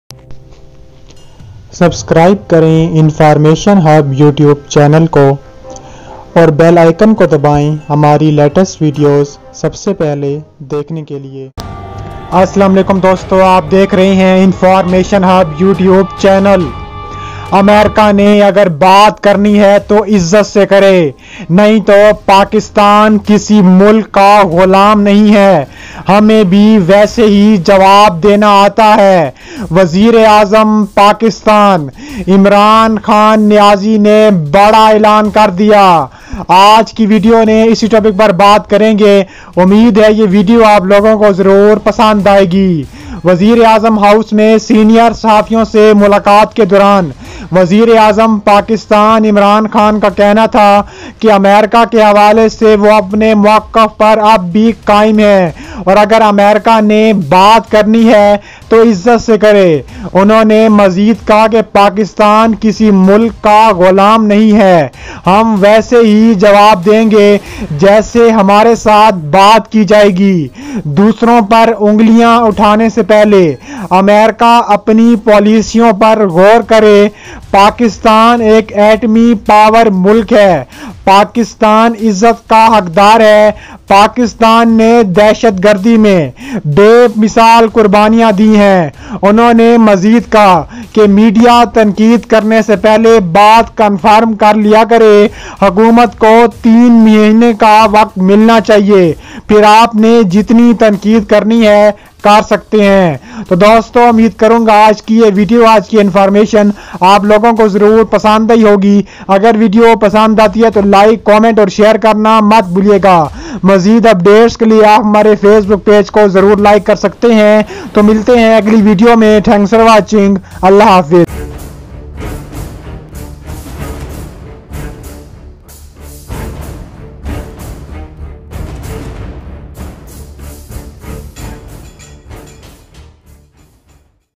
Pakistan Kisi Mulk Ka Ghulam Nahi Hai : Prime Minister
Assalam-o-Alaikum Doston,
Doston Aj Doston Aj Ki Video Main Aapko Batao Ga K Imran Khan ny kaha hai k Pakistan Kisi ki Ghulam Nahi hai.. Agr Video Pasand Aaye To Like Aur Share Zror Kijye Ga and Thanks for Your Time and Support..!!!